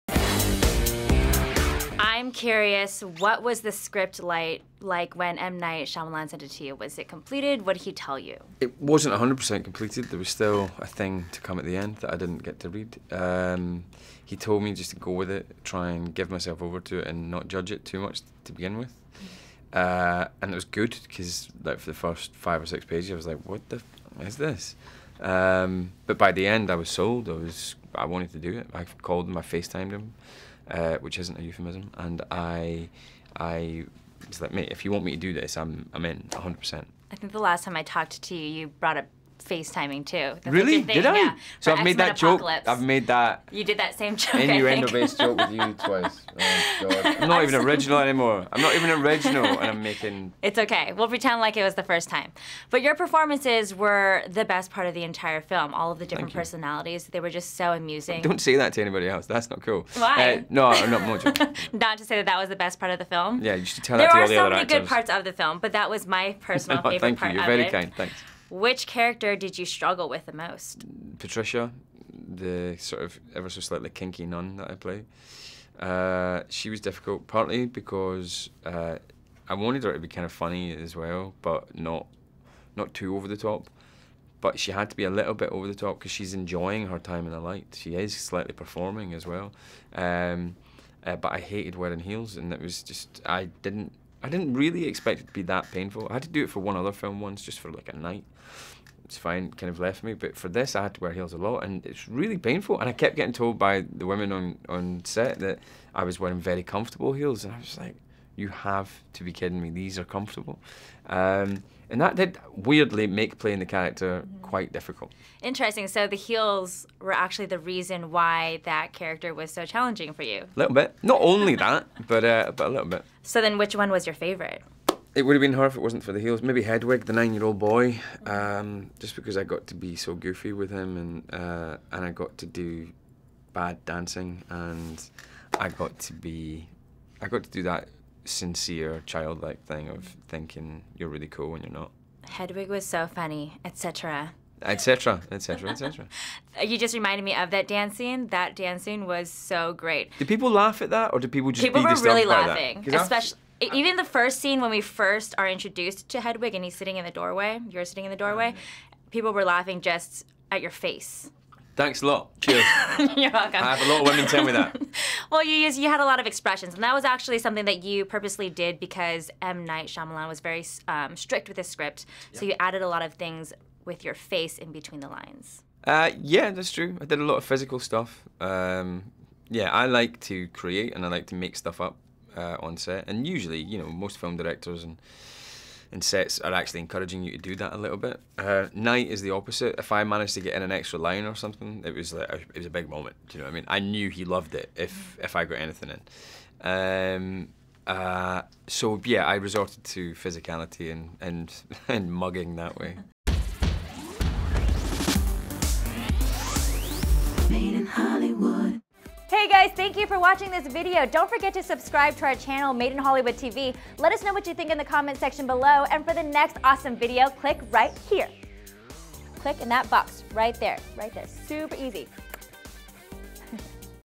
James McAvoy Interview Split
Kylie Erica Mar interviews James McAvoy for Split.

Share this awesome vid!